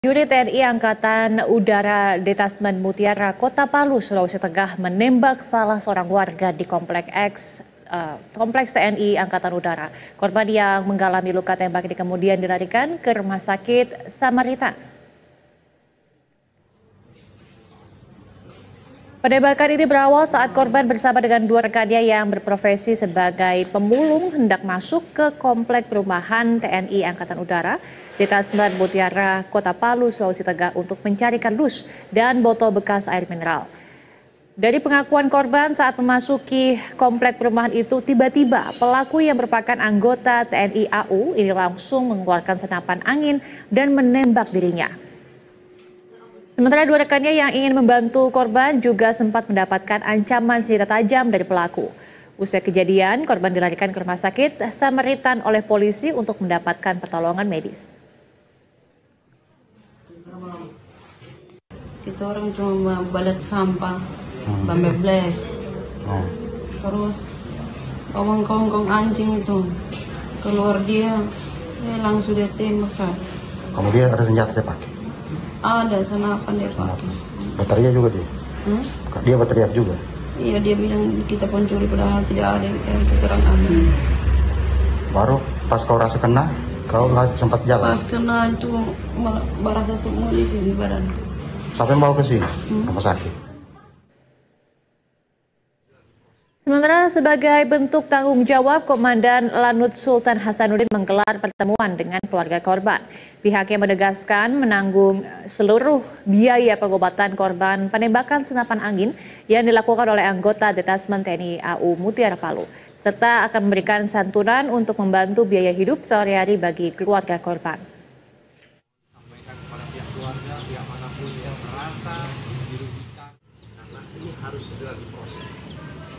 Warga Ditembak Gegara Masuk Kompleks TNI Tanpa Izin | Beritasatu
Seorang prajurit TNI Angkatan Udara Detasemen Mutiara Kota Palu, Sulawesi Tengah, menembak warga di Kompleks TNI AU. Korban yang mengalami luka tembak dilarikan ke Rumah Sakit Samaritan.